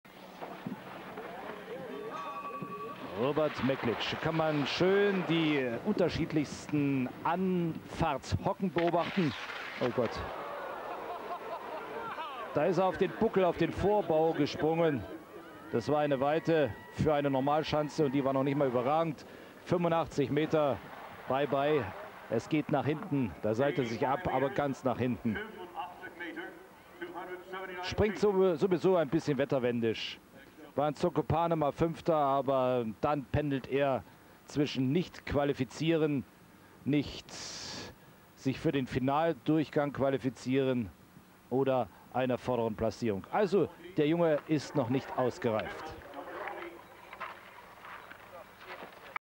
Robert Meglič - 85m - Iron Mountain 18.02.1996 - DOUBLE JUMP [?]
First competition, 2nd Round - 85 meters

Mocne klepnięcie buli słoweńca dzięki czemu przeszurał jeszcze dodatkowe 25 metrów na tyłach nart